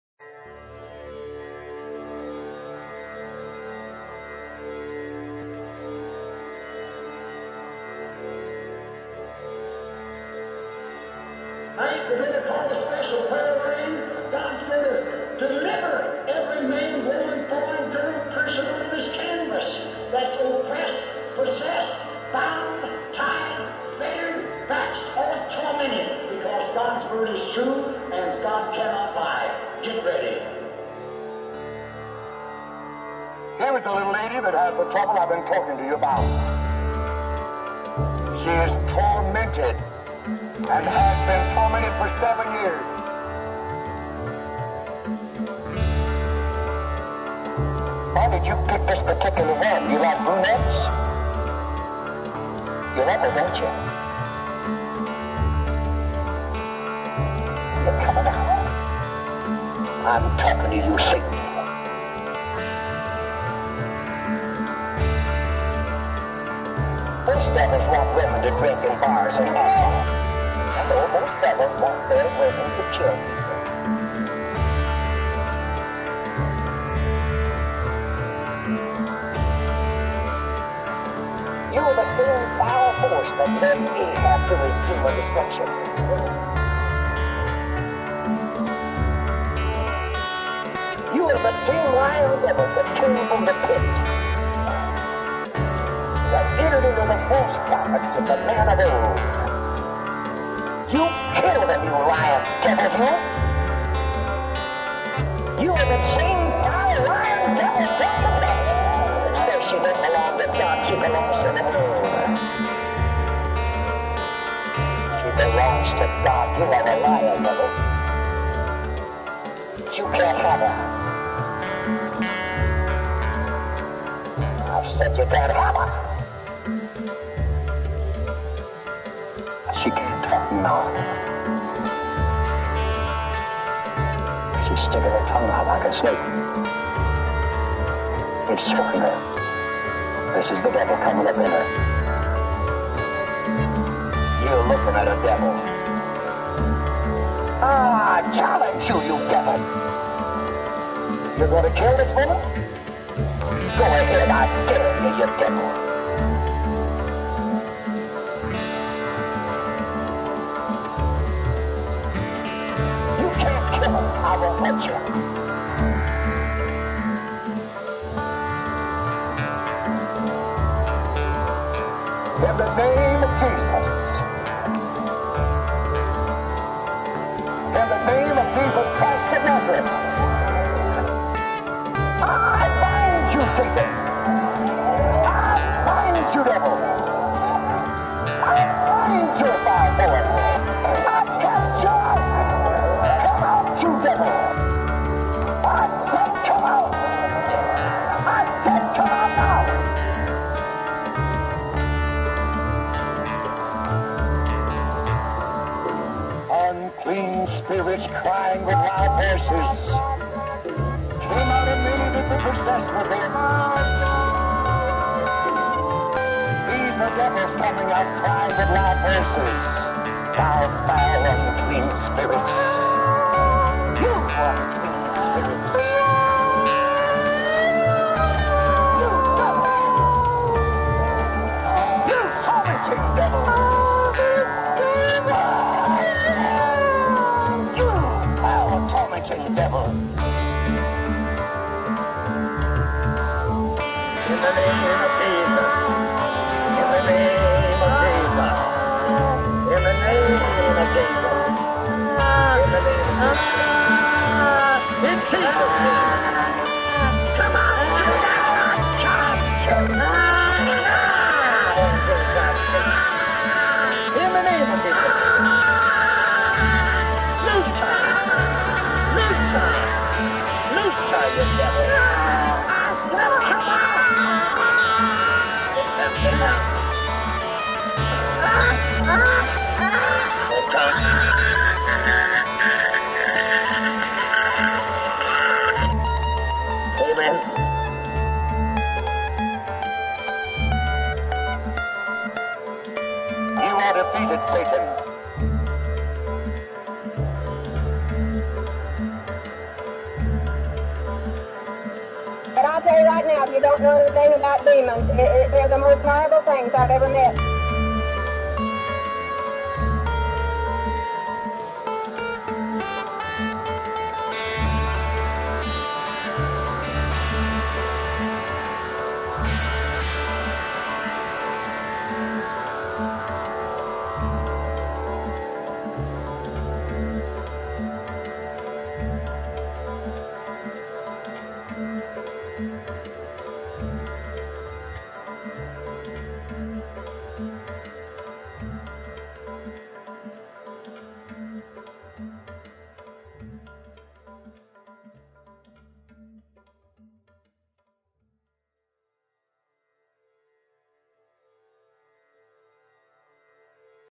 Demon in My View
This video uses sounds and images from demonic exorcisms  from A. A. Allen's revival services in the 1960s, which were documented on television broadcasts and record albums.

The video clips and sounds may be disturbing.

I  also do not want to offend anyone's faith, and I've included some sources at the end of the video that describe Allen's Ministry from different perspectives, in case you wish to learn more.

The sound clips had quality issues- I've done what I can to improve them.  I have also provided Closed Captioning (in English)  which should describe what is said. Any errors in translation are my own.

Most of the music is my playing a Yamaha Acoustic/Electric guitar with Sitar emulation and overdrive pedals, along with percussion loops from Michael Held and freedrumloops.com.